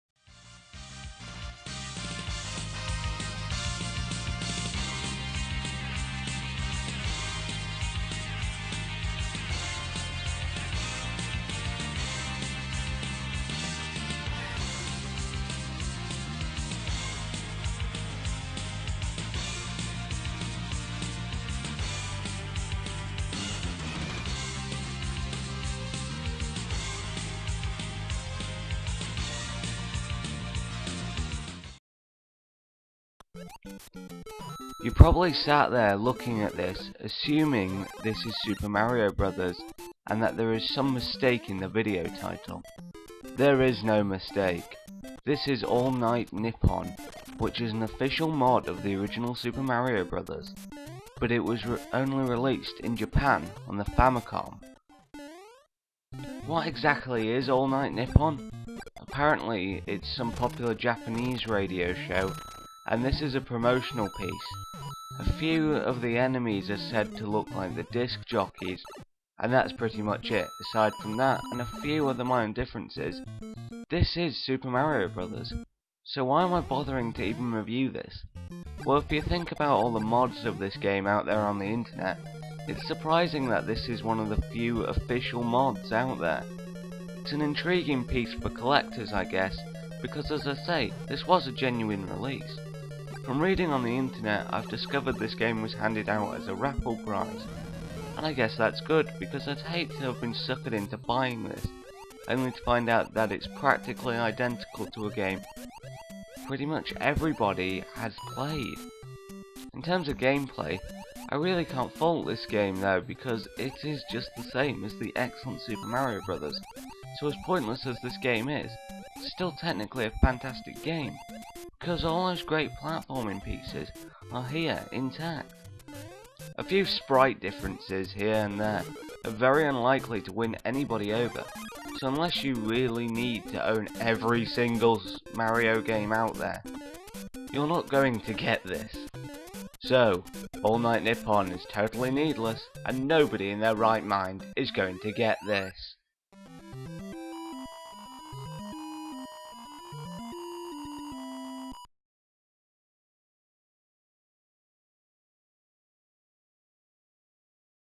All Night Nippon Famicom Review
Joseph of Space Hawk Reviews takes a look at All Night Nippon for the Famicom